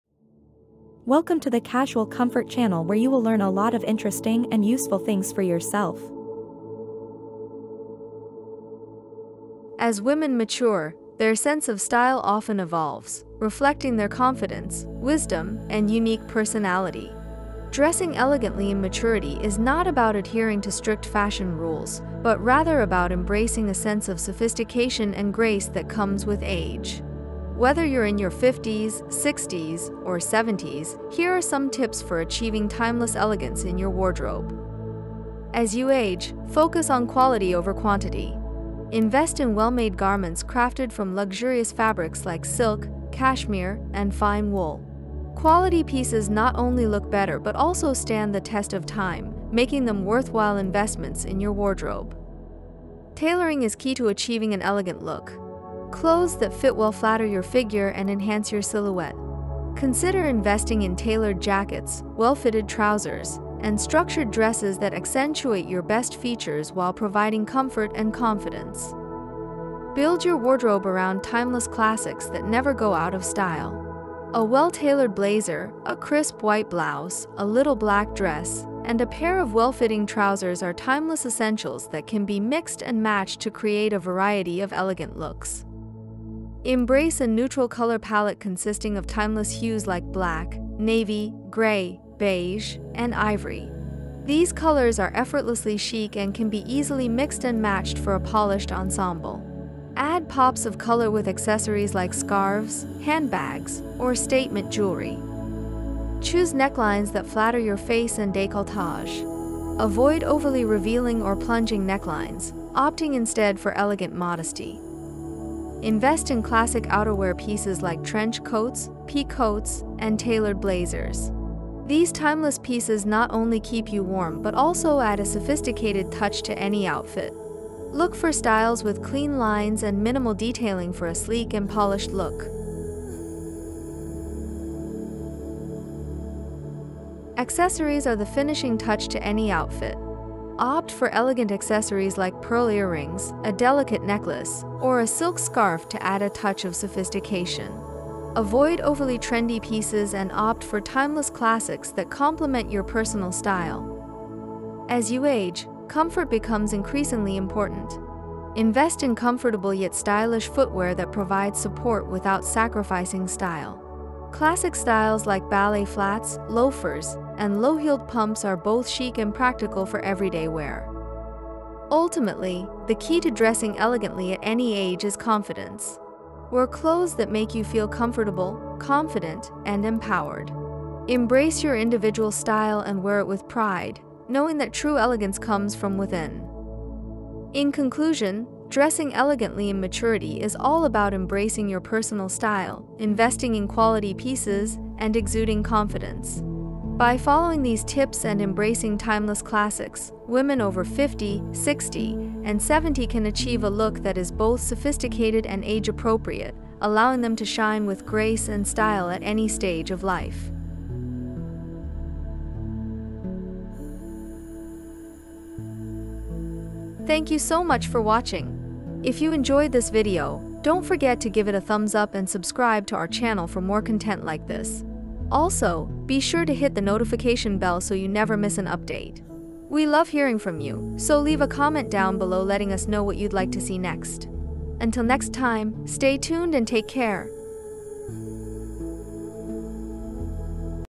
Elegant Over 50s, 60s, 70s: How to Dress Elegantly in Maturity | 2024 Fashion Trends
Age is just a number when it comes to style! In this video, we'll share timeless fashion tips and outfit ideas for those in their 50s, 60s, and 70s who want to exude elegance and sophistication. From classic silhouettes to modern twists, discover how to dress with grace and flair for any occasion. We'll cover flattering cuts, chic accessories, and styling tricks to enhance your natural beauty and confidence. Join us as we redefine elegance and show that style only gets better with age!